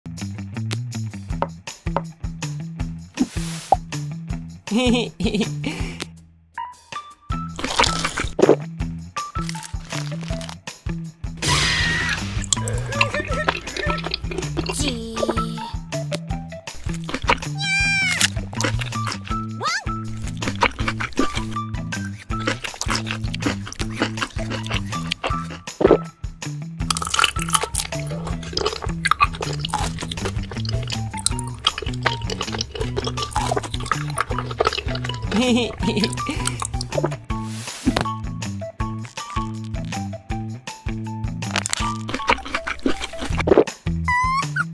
Silver Food VS Real Food Stopmotion Mukbang /스톱모션 애니 실버 햄버거VS 진짜 햄버거 먹방 & ASMR episode 02
Welcome to 먹방 | Mukbang | モッパン | 吃播 Animation Channel!
見てくれてありがとう！ 気に入ったらいいねを残して、コメントで感想を教えてください！ ありがとう。
시청 해주셔서 감사합니다! 즐거우셨다면 좋아요를 남겨주시고 의견에 의견을 남겨주세요! 감사해요.